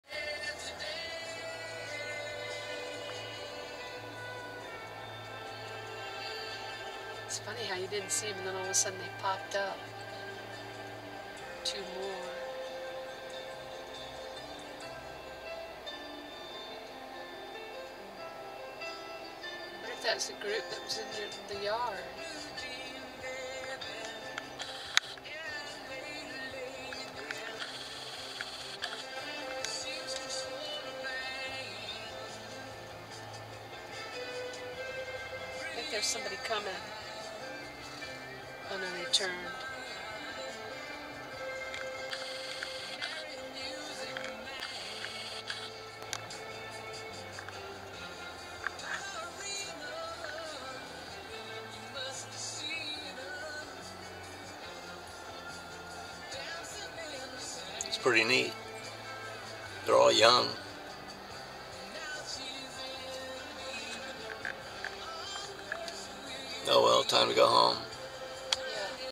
Turkey
Saw these guy's going across the road near the house today on our way home from seeing the storm damage in Cedar Key.